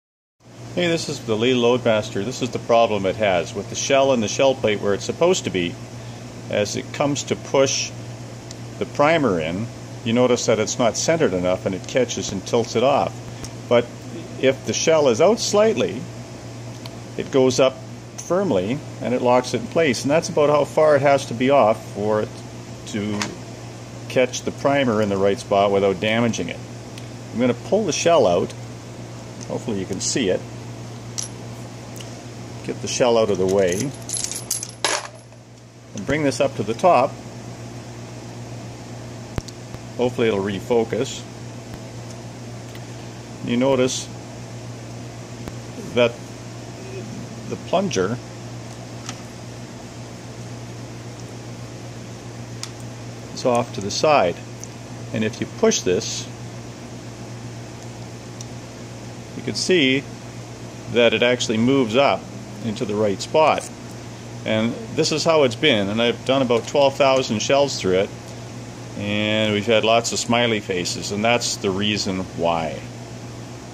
Lee Loadmaster Priming Pin Misalignment
This is my problem with the Primer Pin misalignment for 9mm and 38 super casings. This was most noticeable when switching out to new Starline 38 super casings that seem to have tighter primer pockets; but it may have something to do with parts suffering wear and loosening up (steel versus plastic). Currently I have 12,000 rounds through the press in one year, so I suspect the issue may be due to wear caused by spring pressure outward radially and a rocking point on the primer lever. With the primer spring removed there is a lot of pin play, but the pin does not tilt outward, and gives better results.

At the end of the video you will notice the spring effect holding the pin outward radially from the ideal position. Using the typical die positions, if the shell plate guide was out slightly at the #2 position, the casing could move outward to give reasonable results. However, if you have the guide tight or use a resizing die in this position (#2), it would cause worse dimpling (smiley primer faces), caused by the tilting and misalignment of the pin to primer.

Here is a link to the Primer Crescents showing the improved results with the spring removed

Lee Precision sent me a new Primer Pin, spring, and Small Primer Assembly to fix the problem the next day after I e-mailed them. I will probably leave out the spring to prevent wear to the plastic guide.